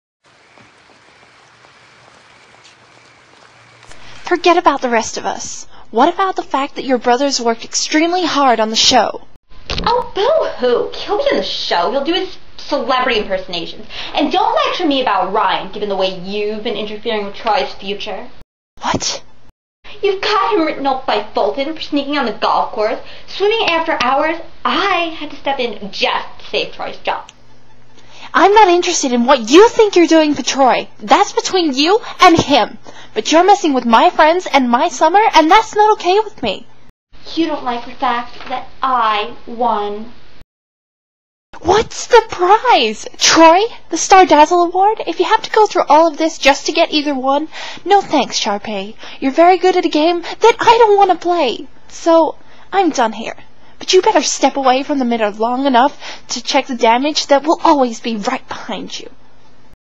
Preview of HSM Voice Series!!!
Okay so I am still working on finalizing the casting.  We are nearly there so just hang in a bit longer well my partner and I figure everything out.  In the meantime I thought I would posy a scene that my friend and I did as a preview of what you will expect from this series.

I am doing the voice of Sharpay

Kristie(xshesallthatx) is doing Gabriella.

Give us your opinions and hopefully the results will be up shortly